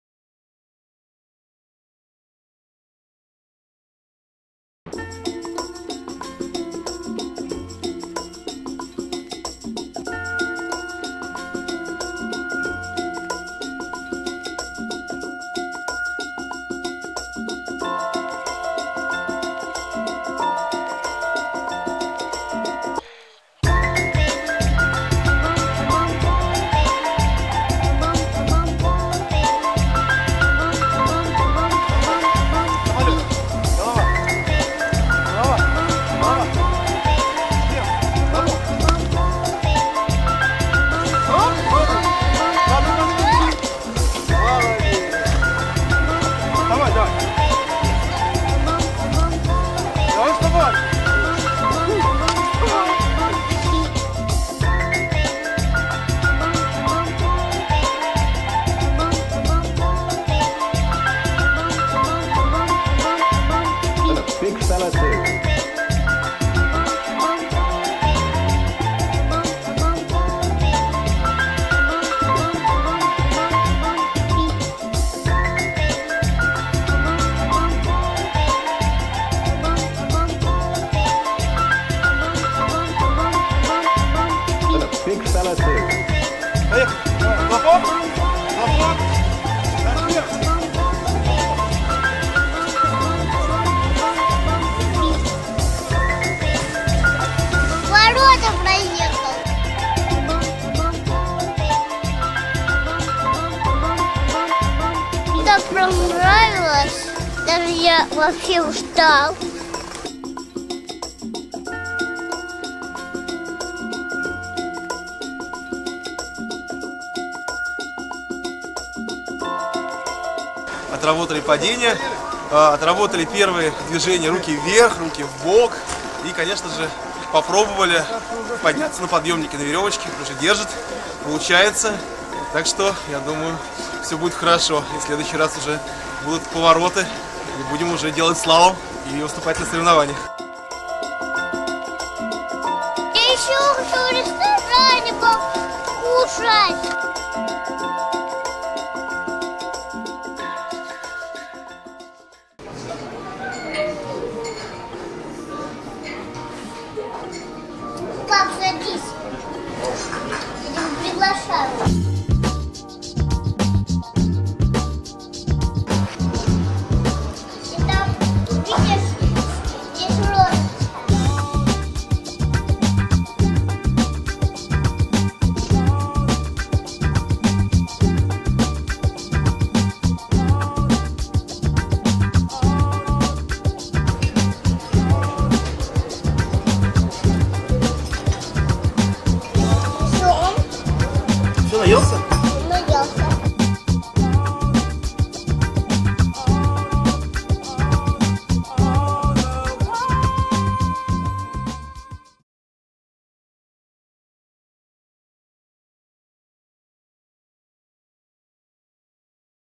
Первый раз на горных лыжах.
Данила учится кататься на горных лыжах. Он это делает в Канте. Здесь есть хорошие инструктора. Каждый инструктор заботится о вашем ребенке как родной отец или...как родная мать! Если он вдруг проголадается на склоне, то в Шале есть много вкусностей для детишек.